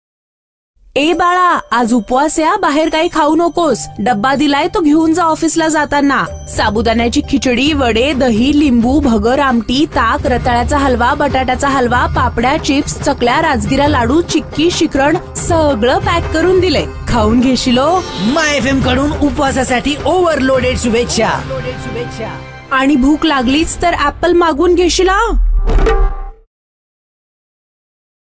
Upwas, Fasting, Don't be Hungry
One eats more on fasting day than a regular day, and still hungry..